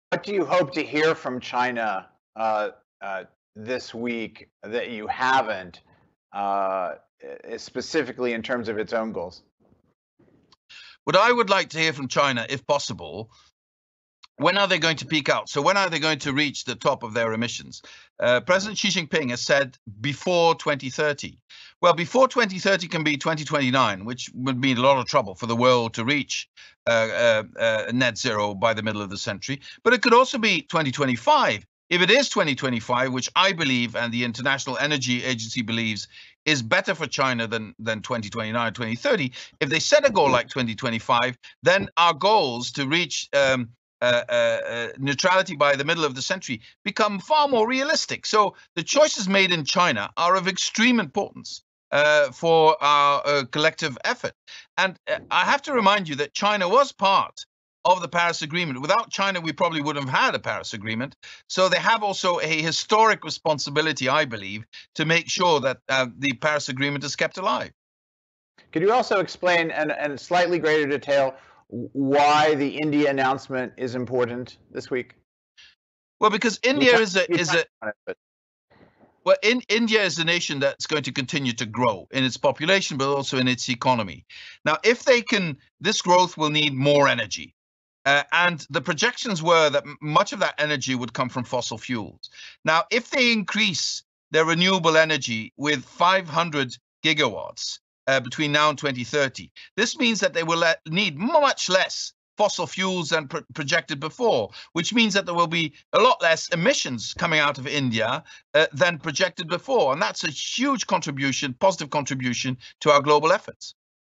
Frans Timmermans says China’s emissions targets are central to world meeting carbon neutrality goals
“What I would like to hear from China if possible… when are they going to reach the top of their emissions? President Xi Jinping has said before 2030. Well, before 2030 can be 2029, which would be a lot of trouble for the world to reach net zero by the middle of the century. It could also be 2025… If they set a goal like 2025, then our goals to reach neutrality by the middle of the century become far more realistic.”